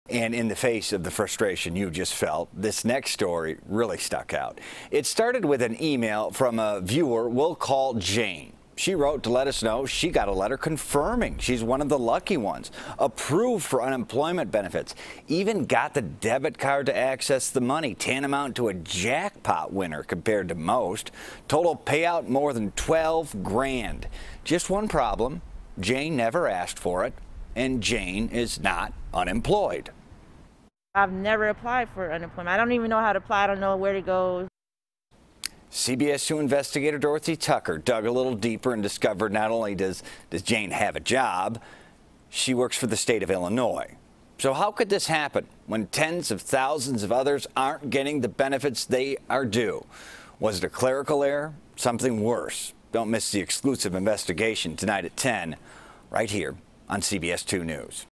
Tonight At 10: Illinois Worker Gets Unemployment Benefits She Didn't Apply For
The state employee is still working and does not need unemployment benefits, but she got them anyway. CBS 2 Investigator Dorothy Tucker reports tonight at 10.